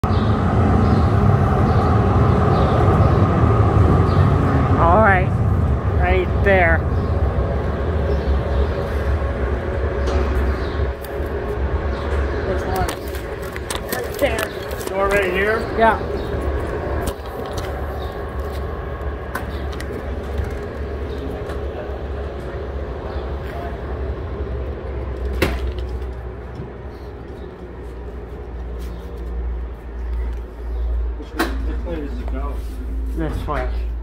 boarding CT Siemens U2 # 2020
have not been on Siemens Duewag U2 # 2020  Car since February 2020  This is another nice treat

Specs year 1982 Siemens U2

Motor Siemens Bush DC